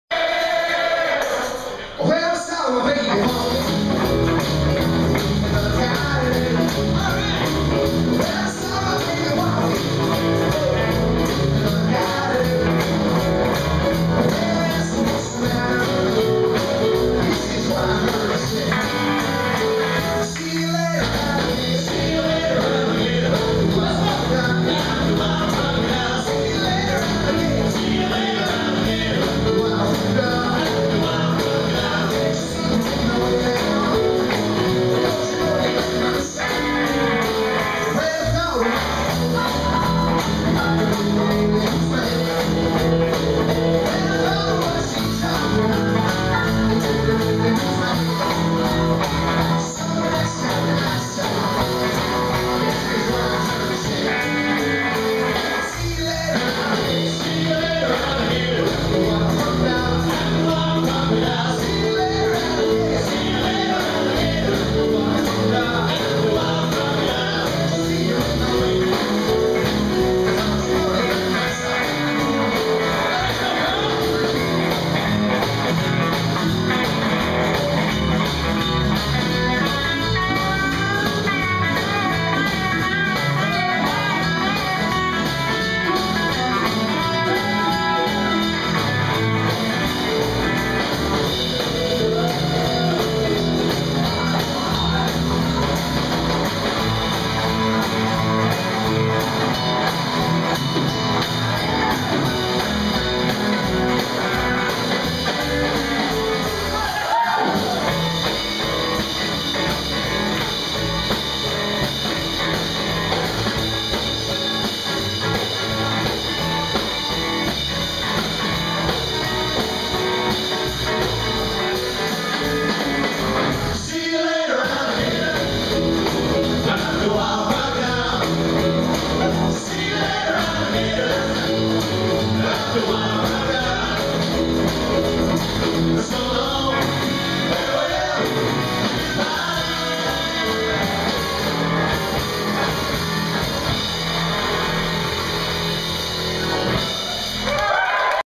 Hard Rock Miami Promo 3
The boys and Dan Mills from the NY cast doing a promo at the Hard Rock Cafe in Miami